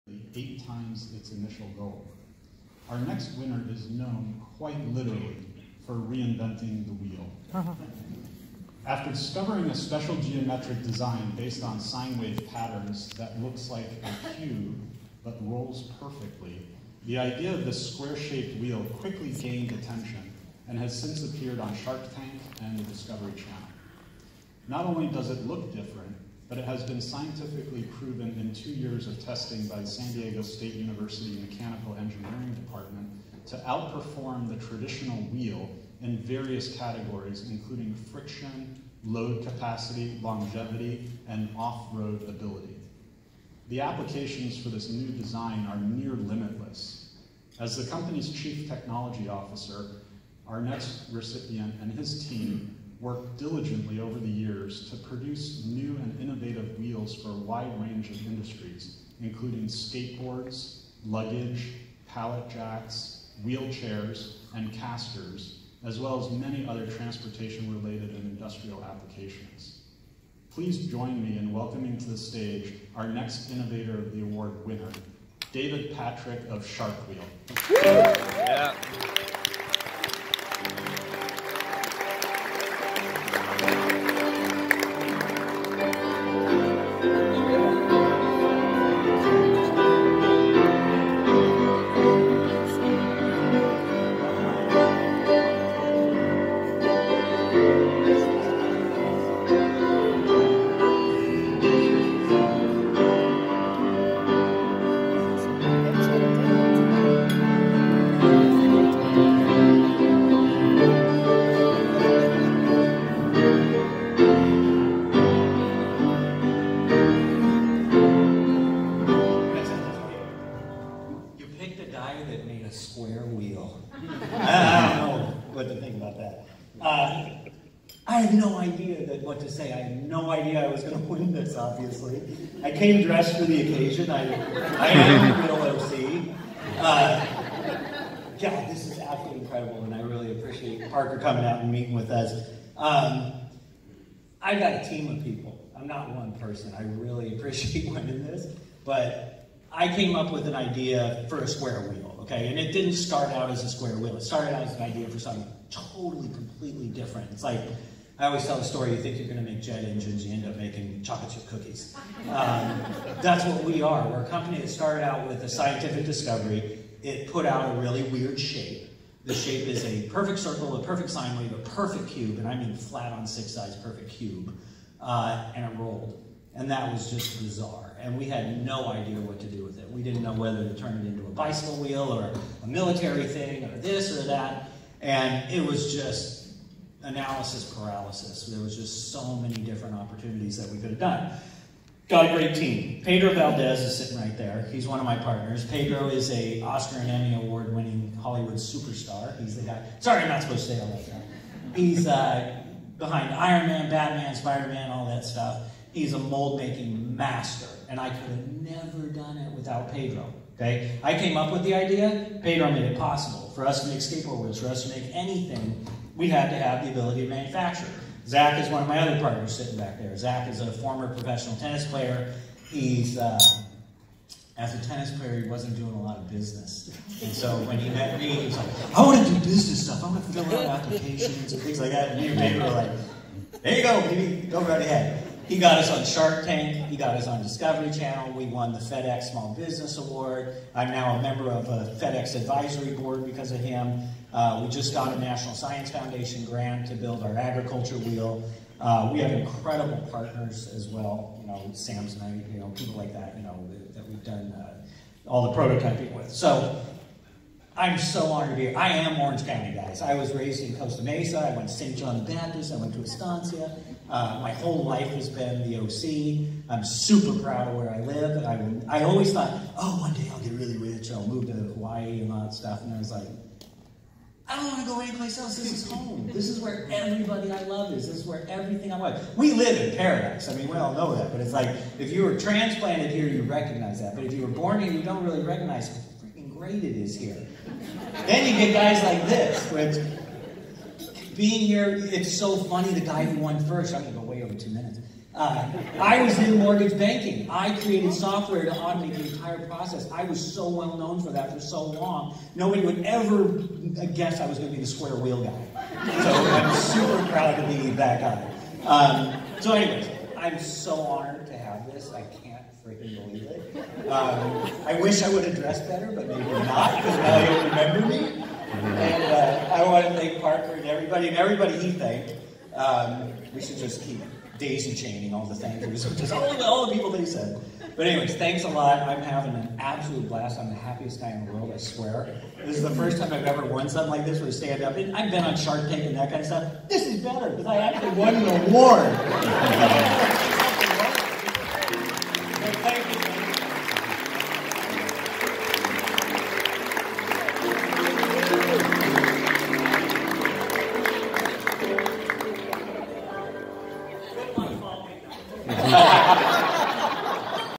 Shark Wheel's David Patrick Wins Innovator of the Year 2018
Orange County Business Journal awards Shark Wheel's David Patrick the Prize

22600 Lambert St. Bldg A, #704